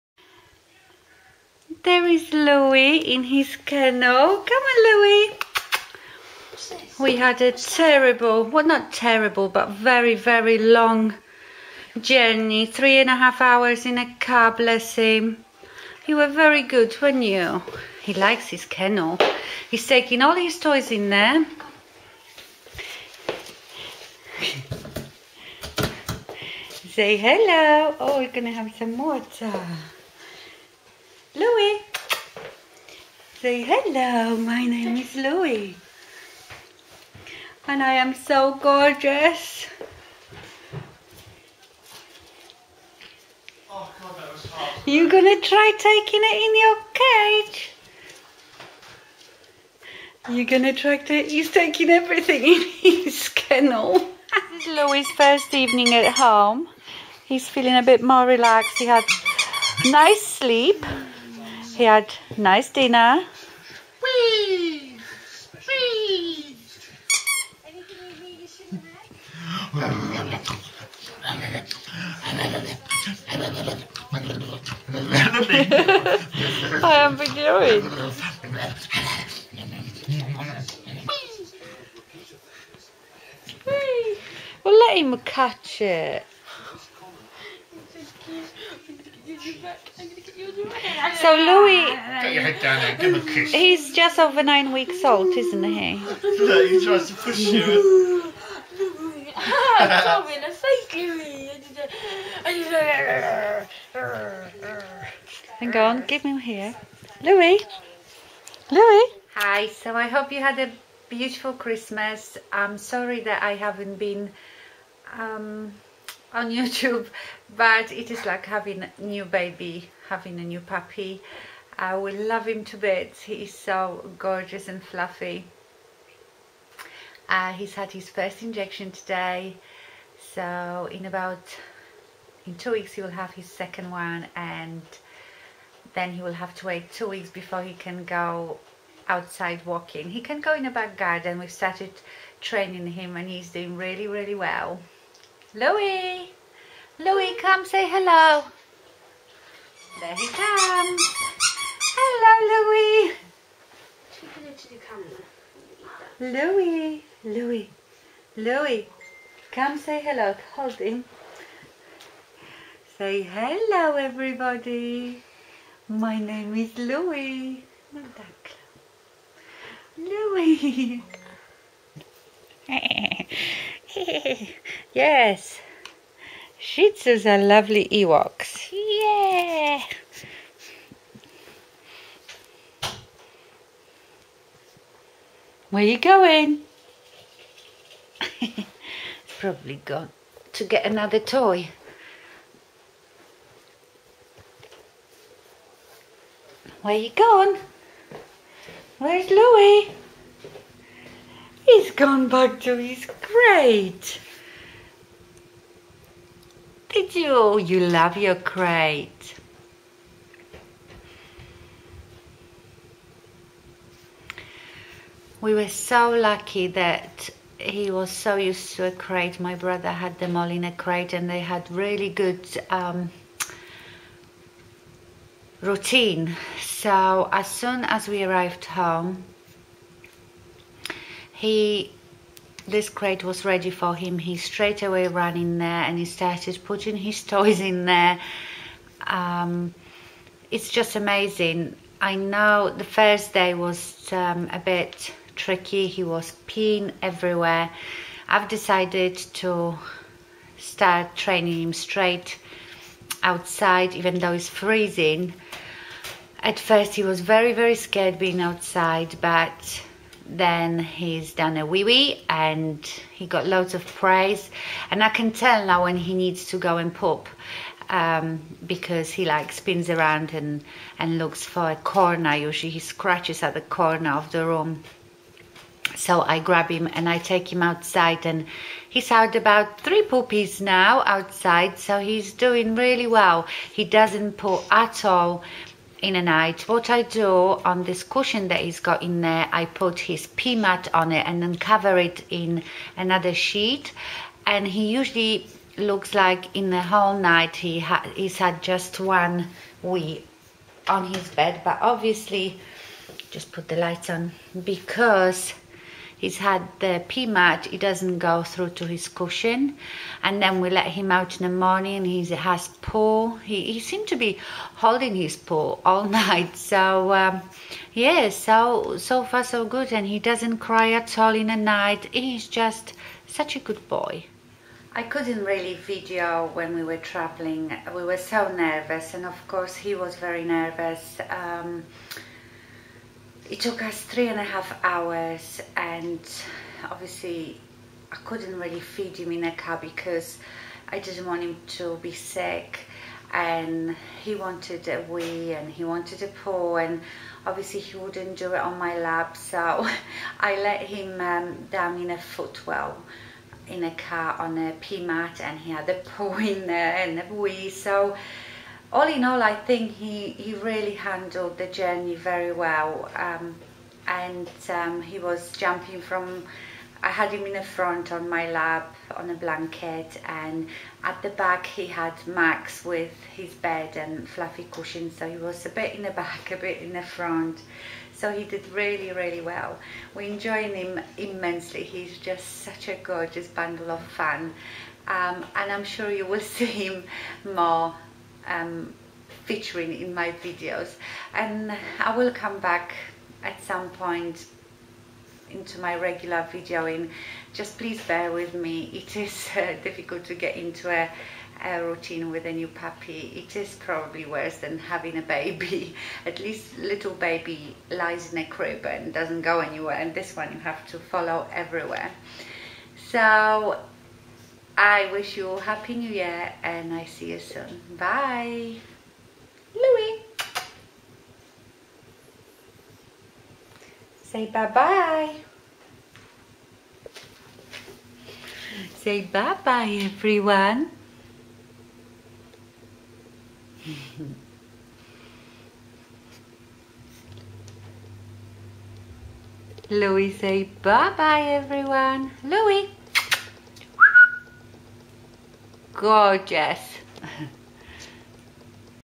MEET OUR NEW PUPPY - 9 WEEKS OLD SHIH TZU LOUIS
My name is Louis and I am a Shih Tzu. I am cute, playful and very clever.